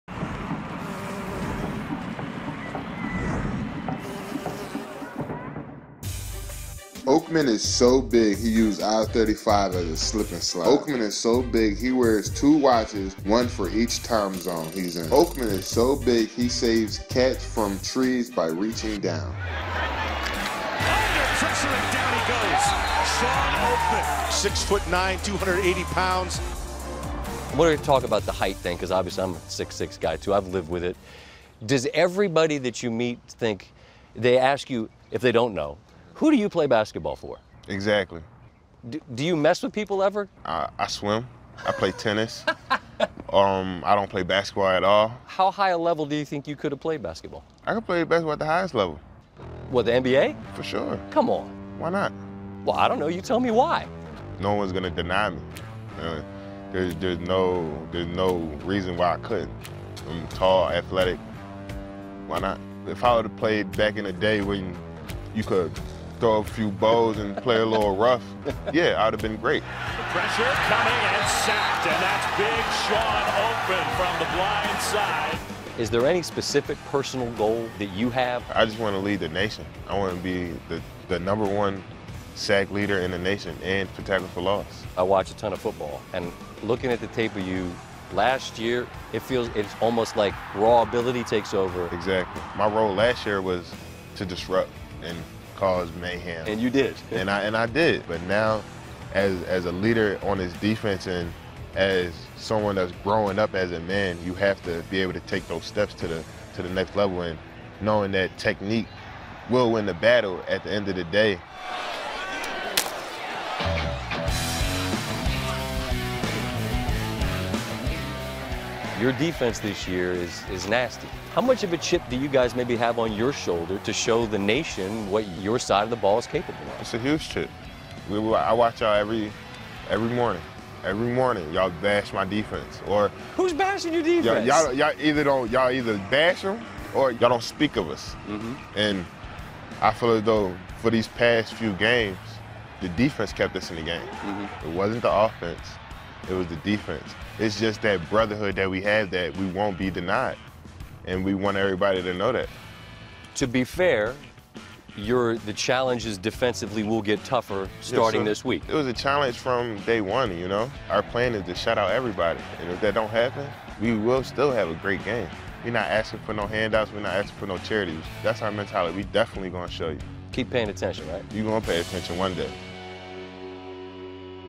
Shawn Oakman oakmanissobig (Full Feature HD)
OAKMANISSOBIG - 6'9, 280lb Baylor DE, Shawn Oakman inspires a trending joke on social media while revitalizing a Bears' defense that leads the Big 12 in sacks.